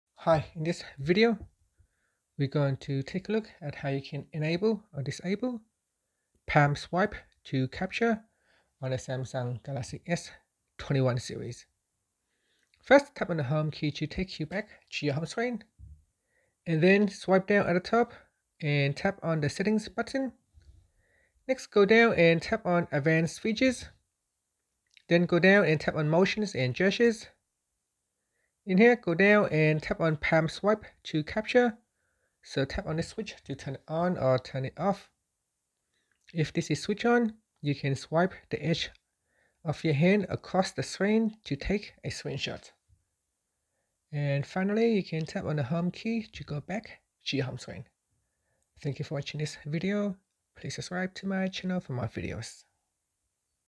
Galaxy S21/Ultra/Plus: How to Enable/Disable Palm Swipe to Capture
Learn how you can enable or disable palm swipe to capture on the Galaxy S21/Ultra/Plus.